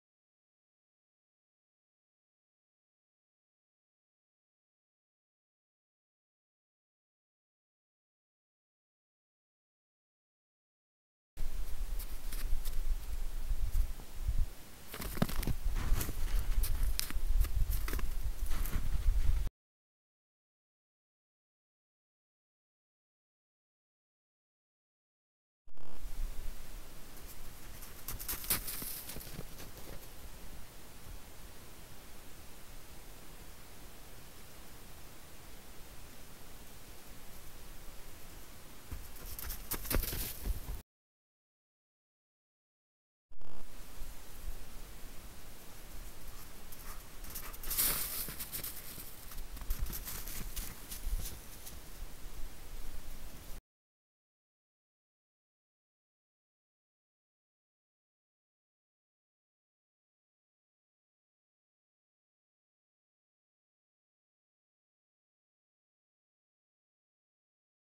MomentsInNature P35 Wolf Chasing Deers
In this episode of moments in nature trail camera was "hunting wolves" in an area that has lot of deers during winter, and some wolves of course. First something scared couple of deers out of their normal trail, but the reason was a mystery, but couple of nights later that changed...